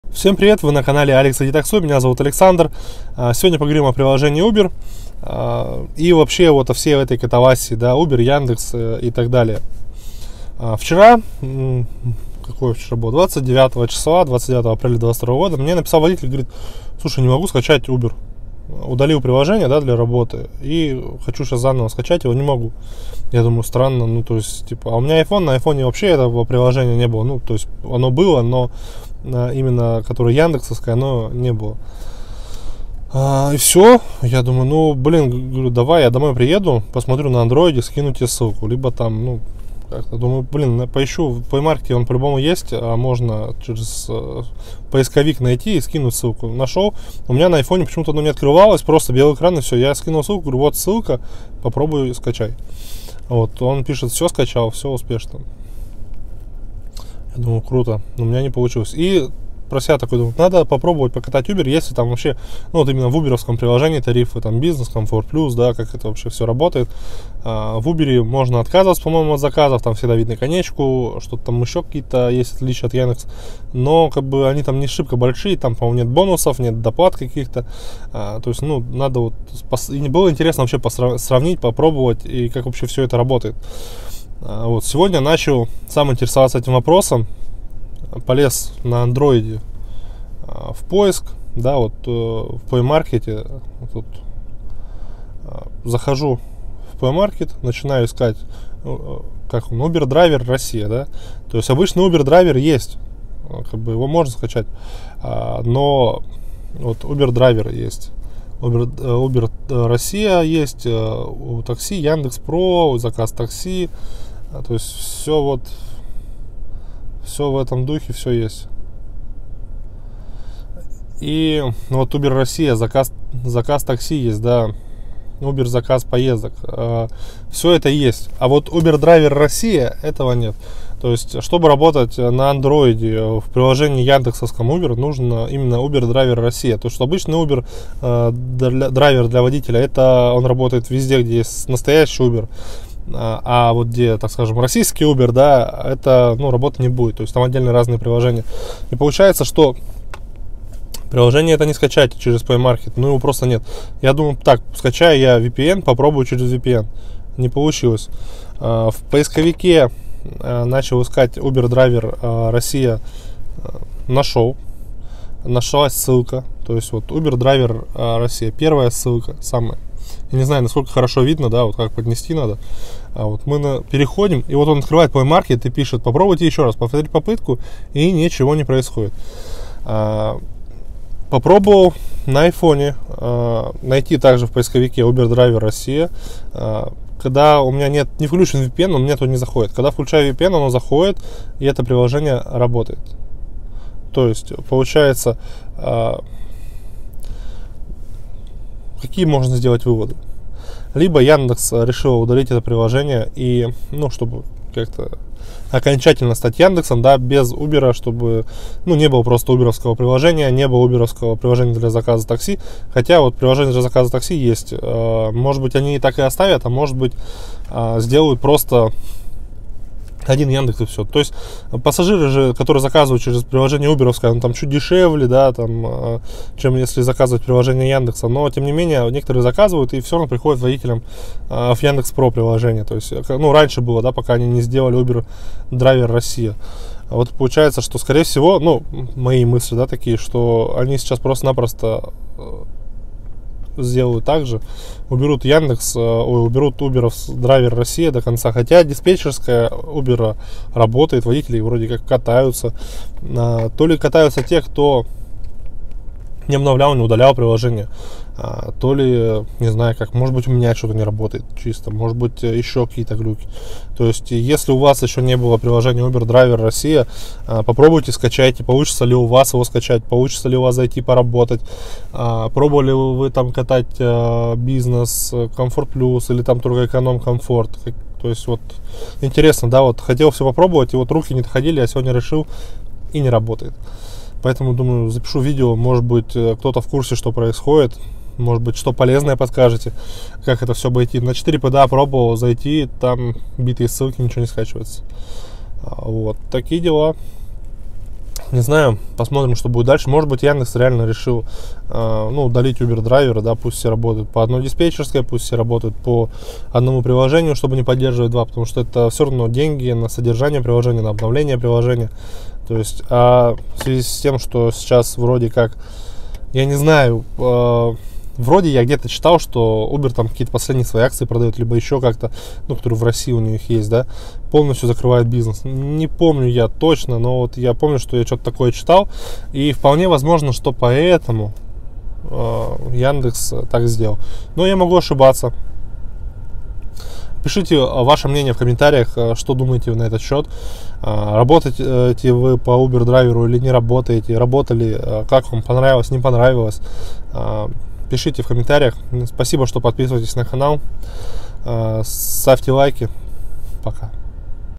Uber Driver Russia - все ?
Всем привет, меня зовут Александр 😌
Взял машину в аренду для наборы в такси в Новосибирске.